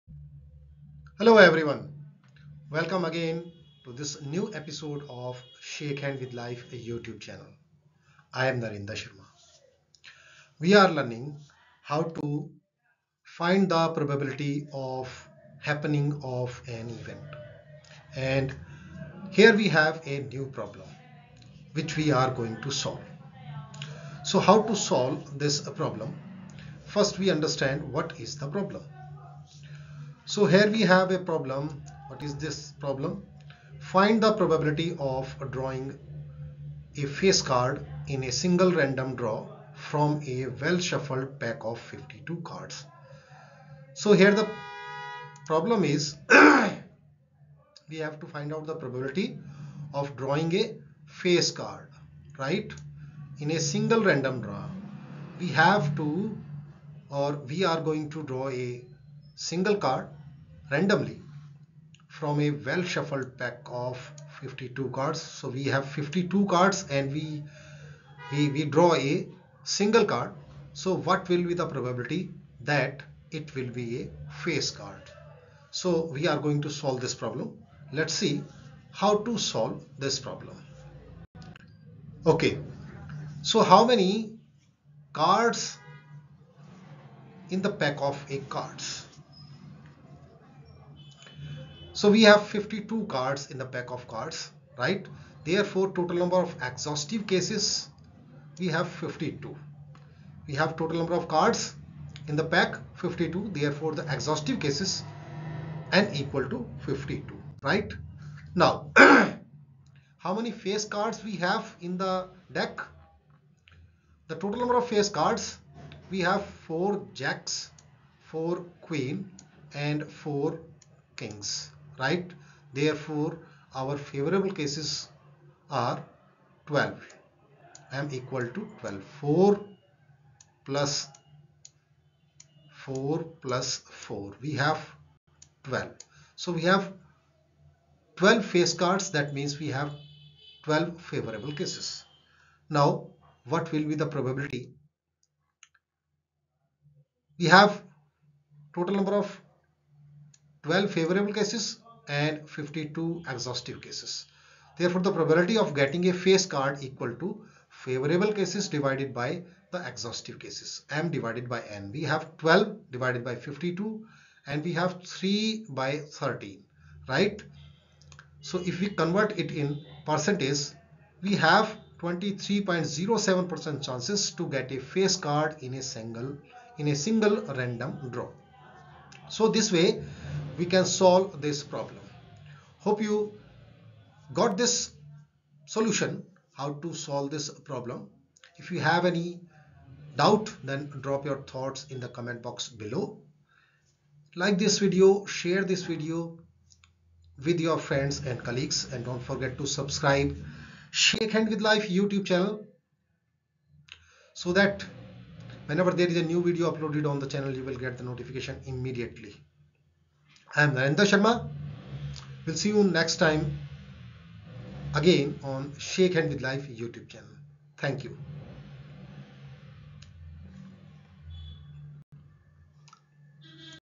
PROBABILITY of Getting A FACE CARD from A Pack of 52 Cards | Probability and Statistics
PROBABILITY of Getting A FACE CARD from A Pack of 52 Cards | Probability and Statistics @ShakehandwithLife

0:00 Introduction
0:11 Problem

SOLUTION
1:21 Total No. of Exhaustive Cases (Number on Cards)
1:47 Favorable cases (Number of face cards)
2:24 Probability of Getting a face card

►Probability of Getting a Red Ball from a Bag of 5 Red and 4 Black balls